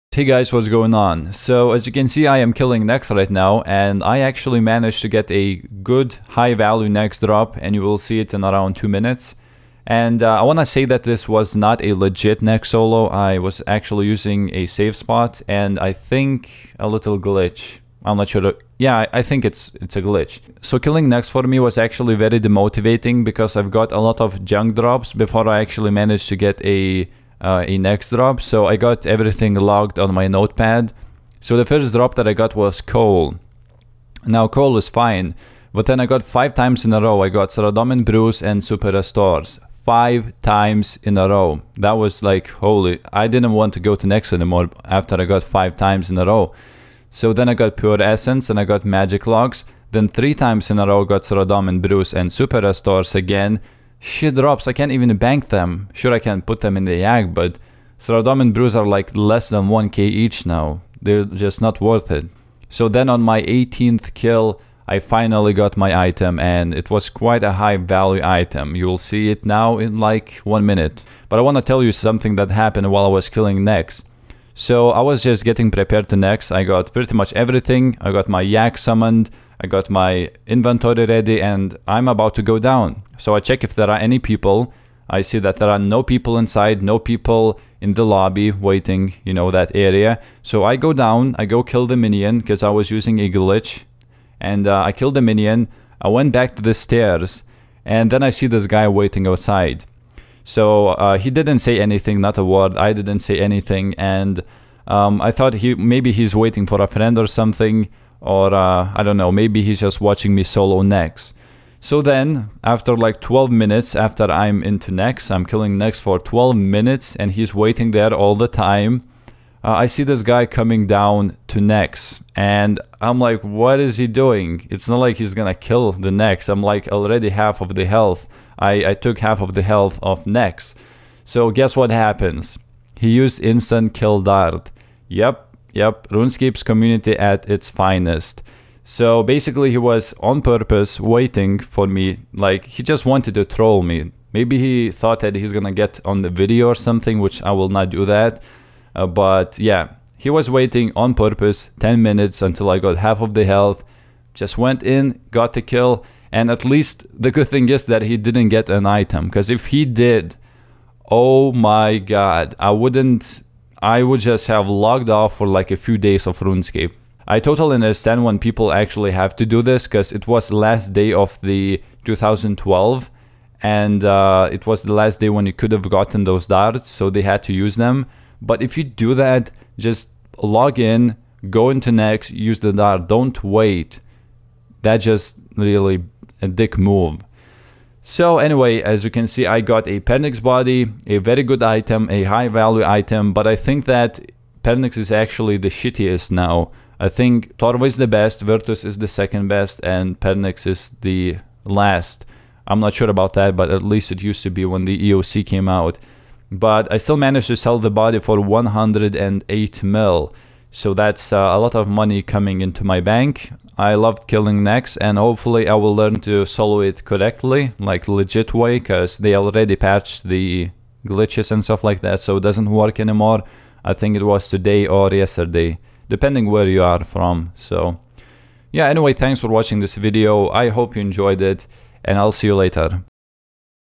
Surprise Nex solo drop #2
Bank was made. I was told that Nex safe spot was fixed yesterday but apparently they lied -.- you can still poison it.